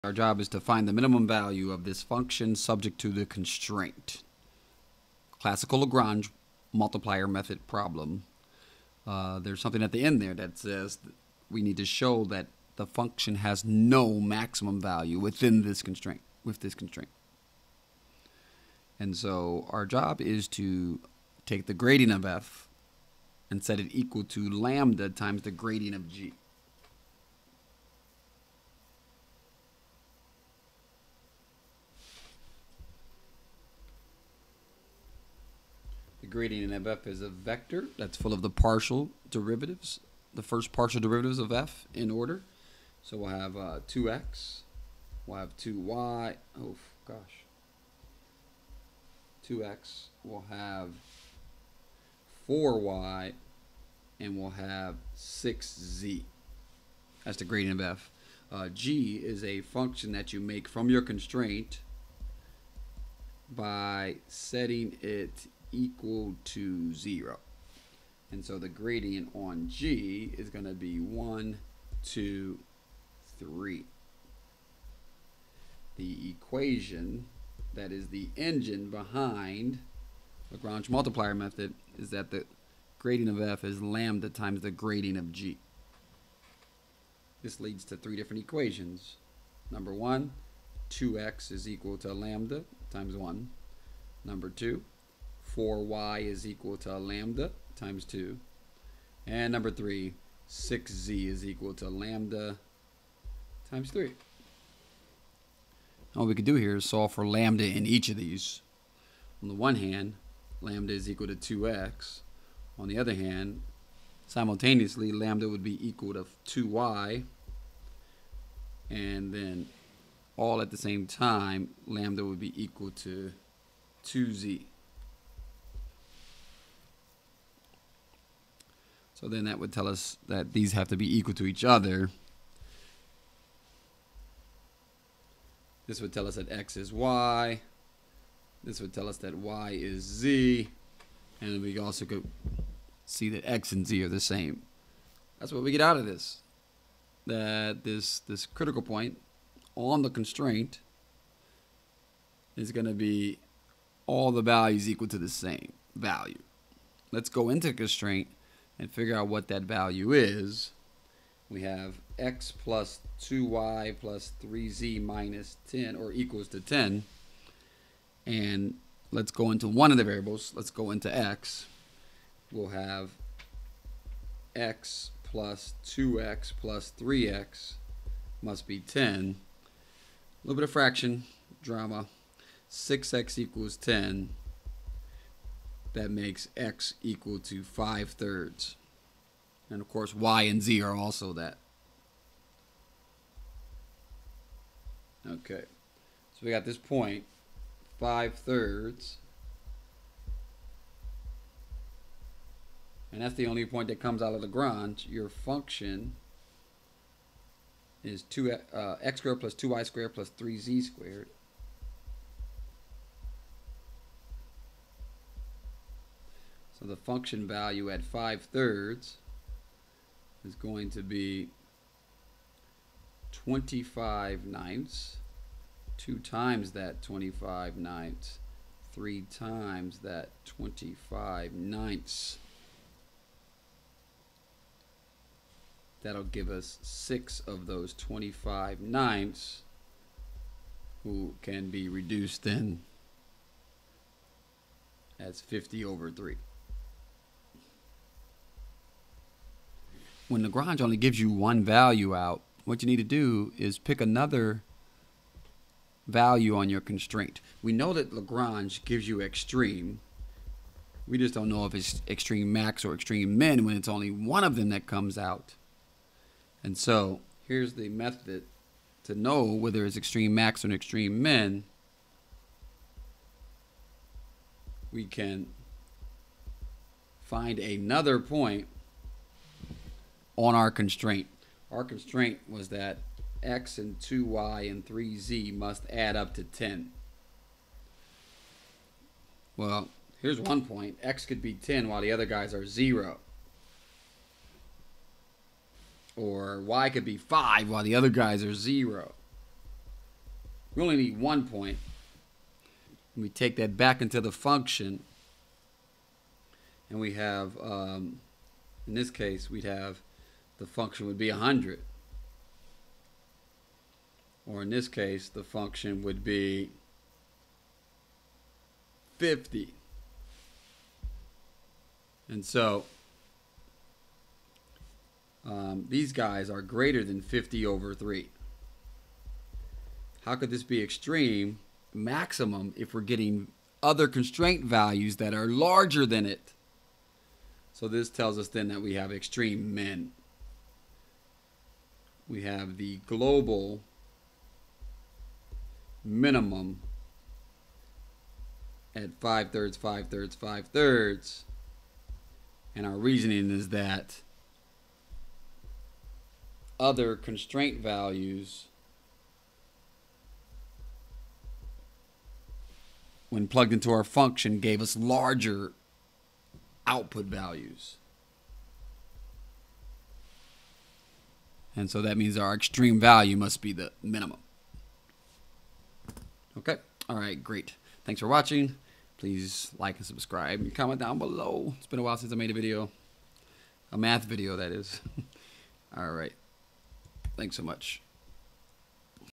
Lagrange Multiplier Method Example
Optimize an objective function subject to a constraint function. This is solved using the Lagrange multiplier method. We are guaranteed to get an extreme value, but what happens when we only get one value out of the method? We explore this question in this example video.